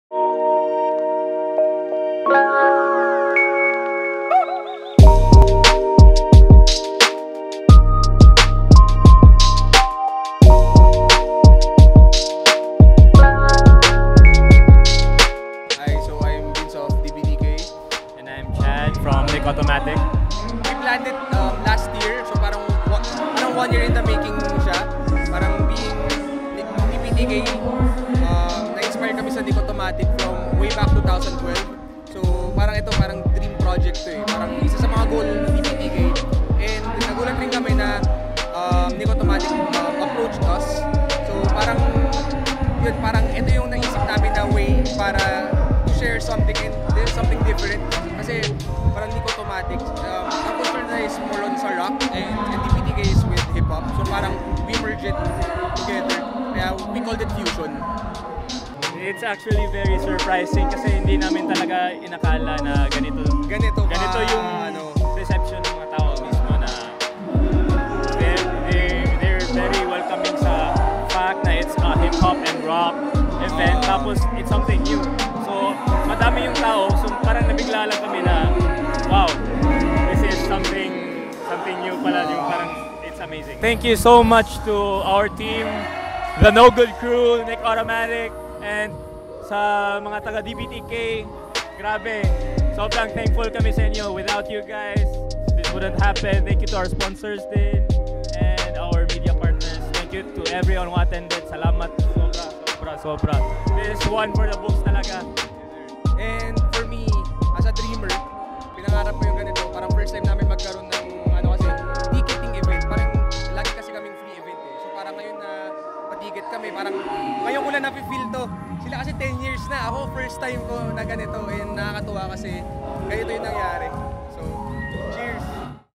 DBTK x Nick Automatic: FUSION | Event Highlights
Did you miss the legendary collaboration event between popular indie apparel brands Don't Blame The Kids and Nick Automatic? Here's a look at the event's highlights and an exclusive interview with the people behind it.